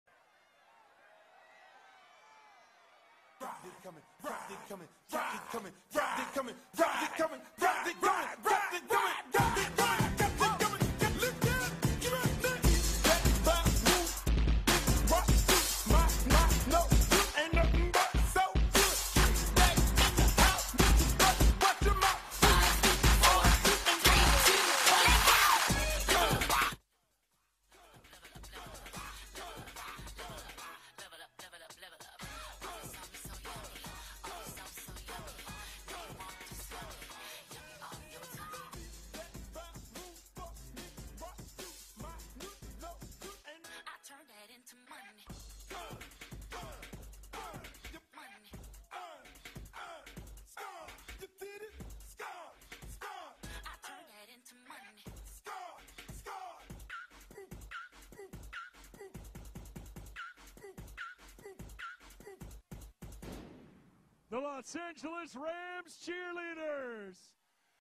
Raw Video: Rams Cheerleaders Perform At Team's Super Bowl Send-Off Rally
The Los Angeles Rams Cheerleaders appeared during the team's send-off ahead of next Sunday's Super Bowl.